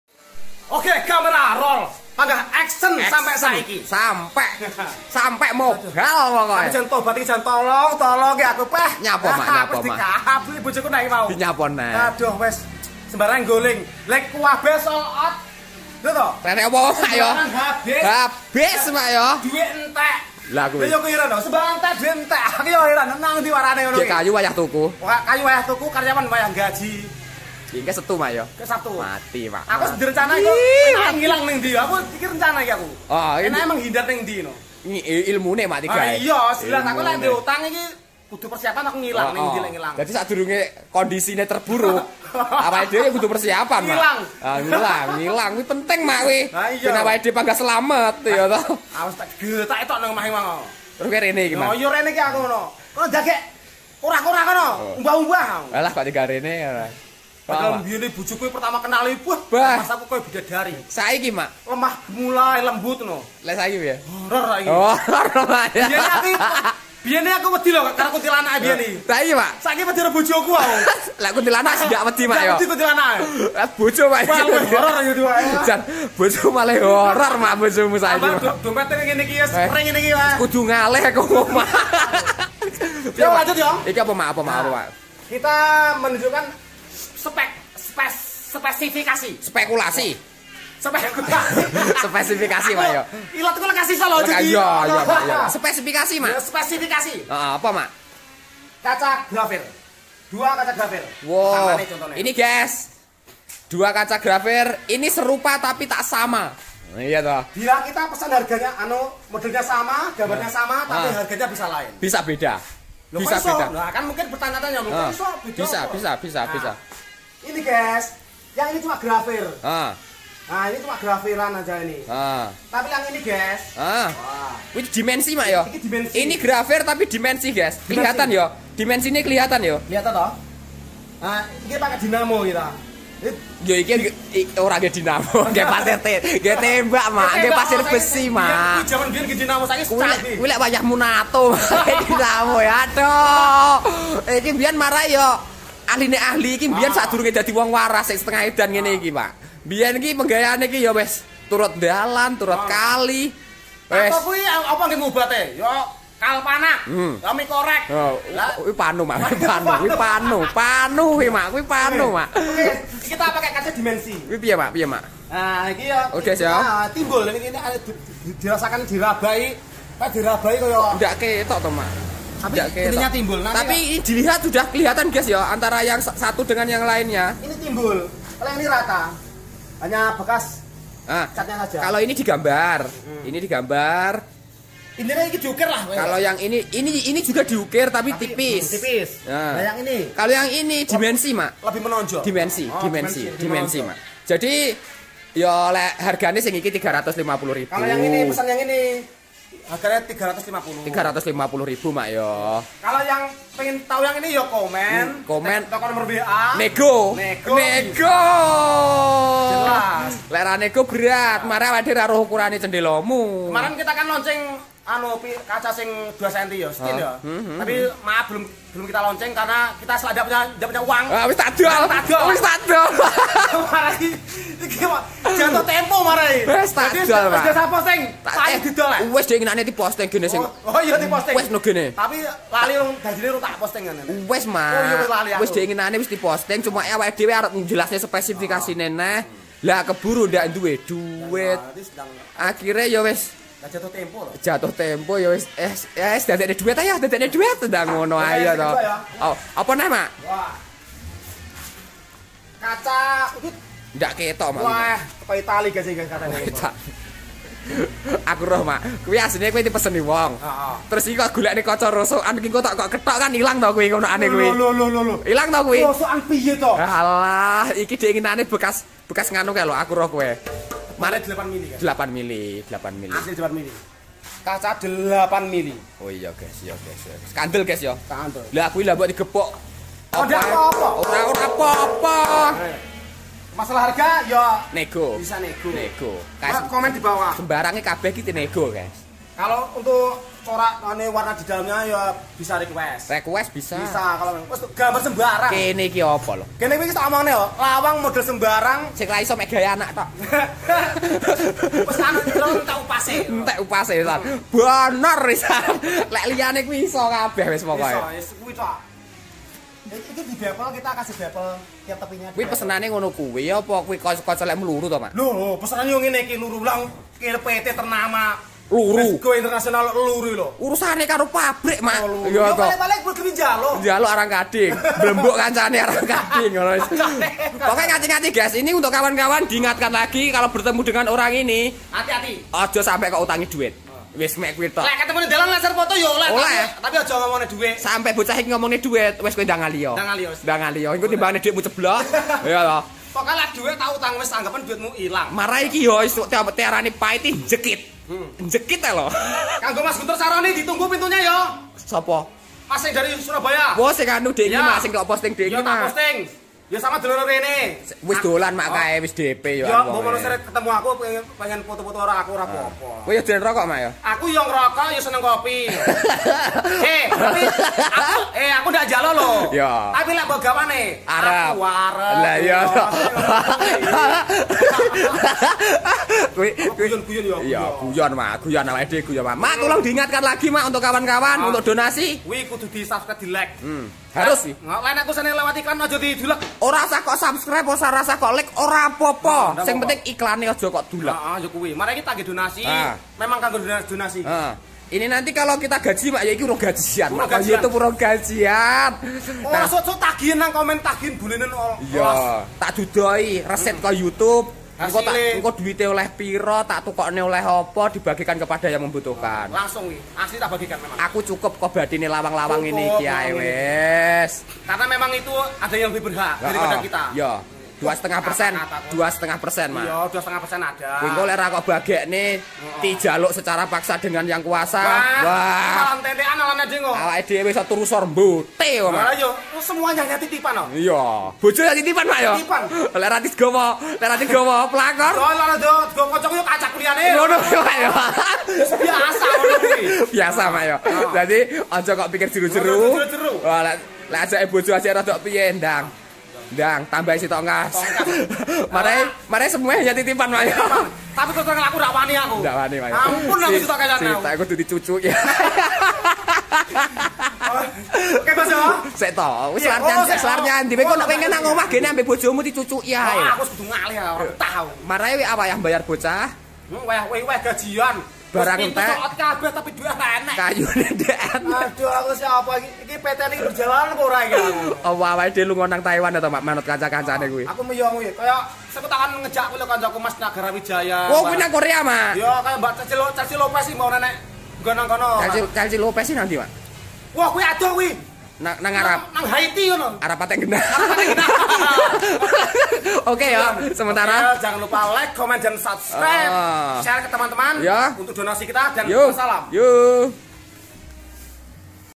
Perbedaan dan spesifikasi kaca hias biasa vs kaca hias dimensi
segera miliki untuk kenyamanan rumah anda
Kami menyediakan dengan harga yang sangat terjangkau